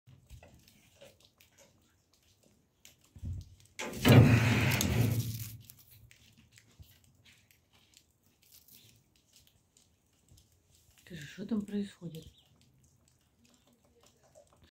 Волк прибывший из Вязники, Владимирской области
Забраны из армянского передвижного зоопарка